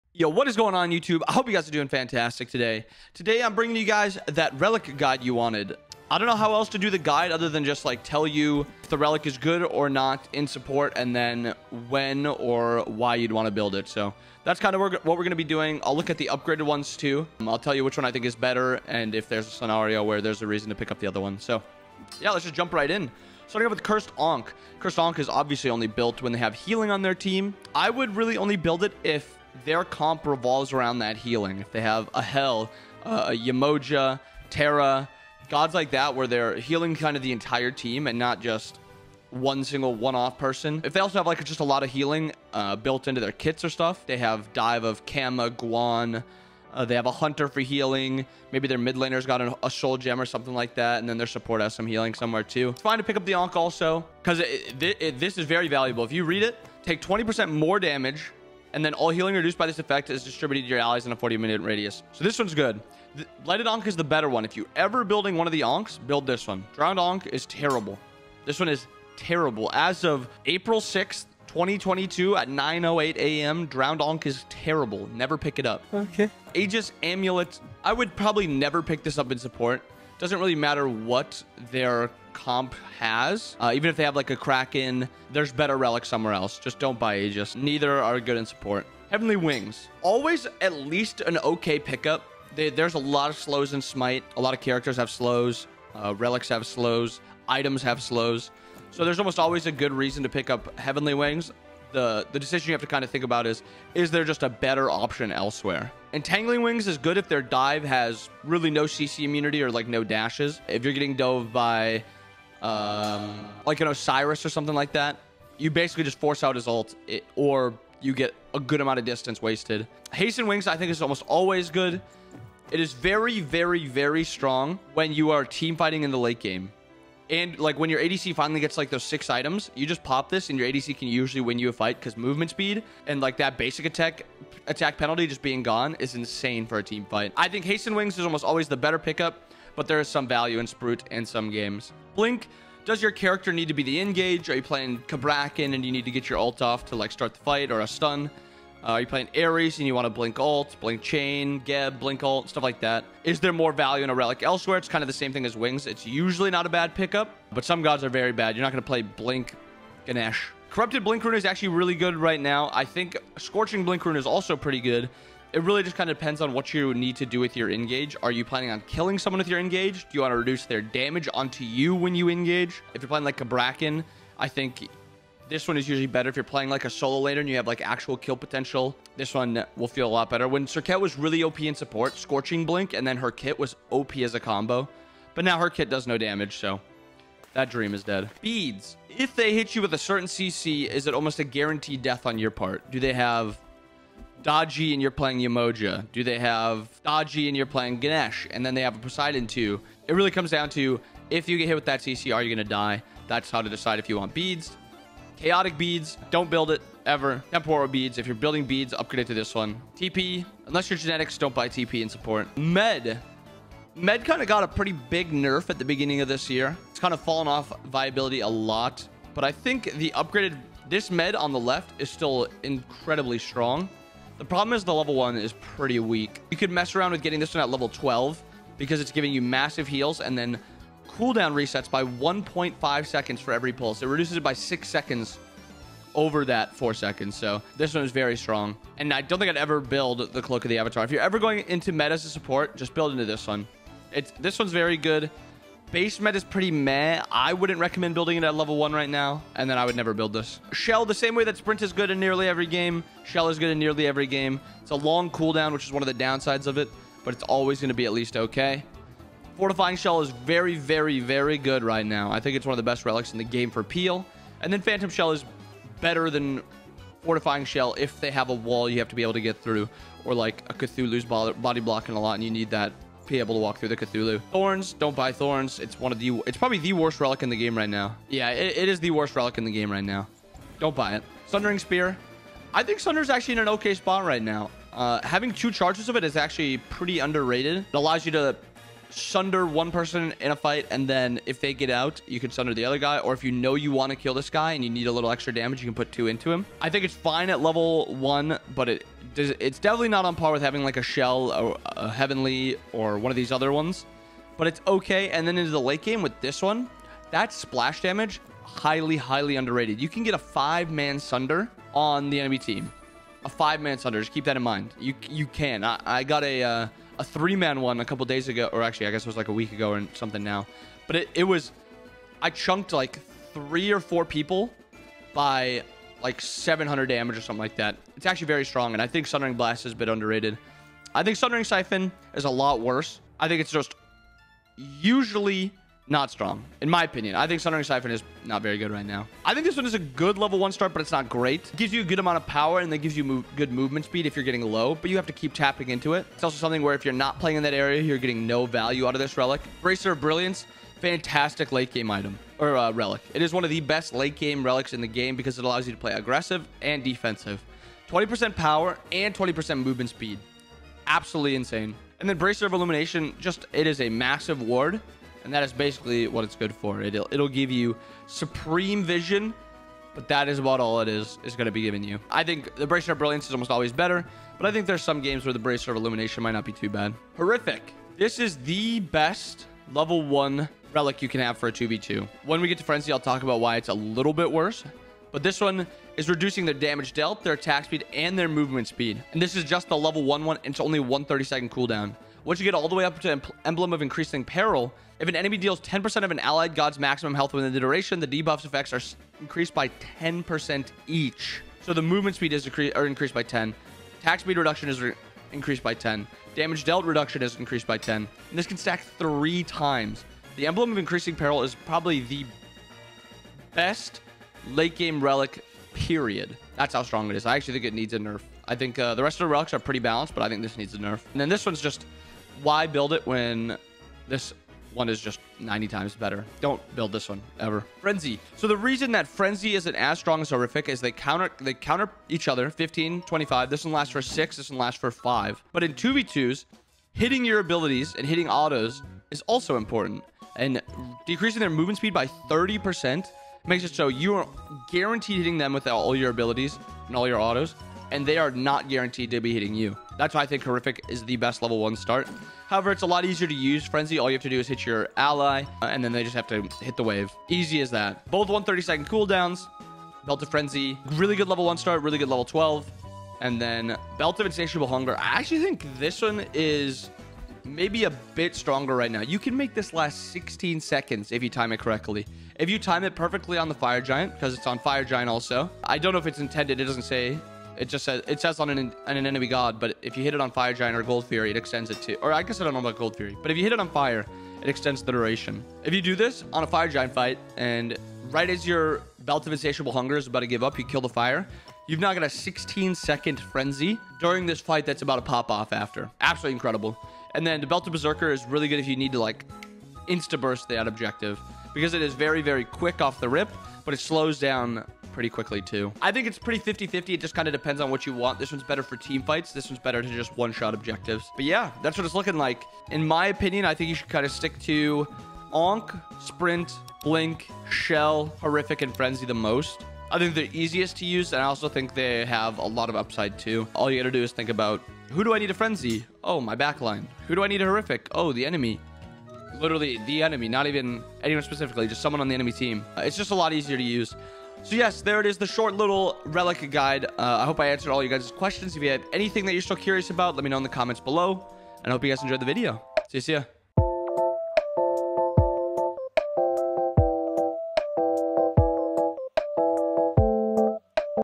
THE ULTIMATE SUPPORT RELIC GUIDE! What should I build level 1? WHICH UPGRADE? ALL QUESTIONS ANSWERED
A lot of you all have been asking for a support relic guide, so here we are. Which relics are best? When should I build each one? Which upgrade should I go? I give all of that information in today's video!

0:00 - Intro
0:23 - Cursed Ankh
1:17 - Aegis Amulet
1:29 - Heavenly Wings
2:22 - Blink Rune
3:09 - Purification Beads
3:30 - Teleport Fragment
3:33 - Cloak of Meditation
4:11 - Magic Shell
4:34 - Shield of Thorns
4:41 - Sundering Spear
5:39 - Bracer of Radiance
6:18 - Horrific Emblem
7:32 - Belt of Frenzy
8:31 - Final Thoughts